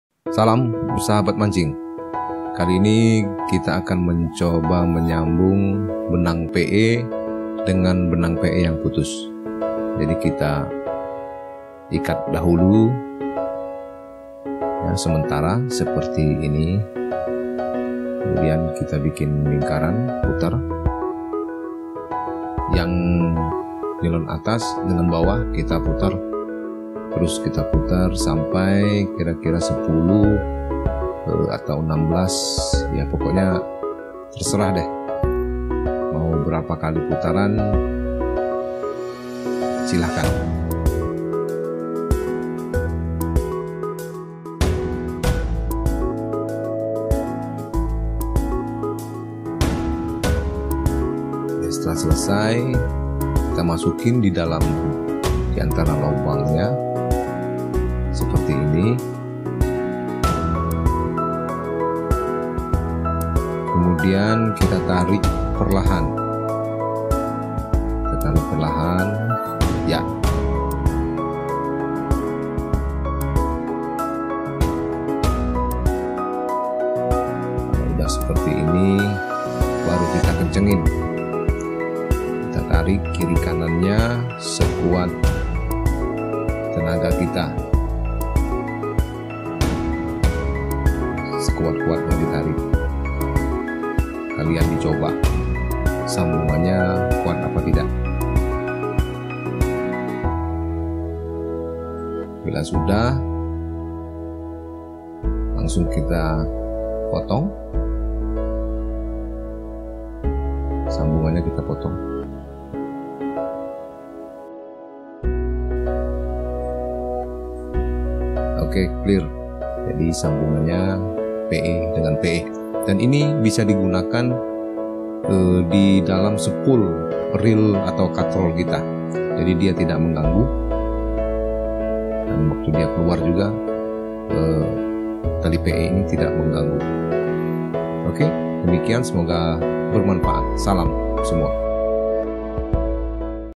CARA MENYAMBUNG TALI PE DENGAN PE DIJAMIN KUAT
cara ini biasa digunakan disaat kita mancing di kapal, atau saat benang pe putus di kondisi yang membutuhkan sambungan dengan cepat.
semoga bermanfaat.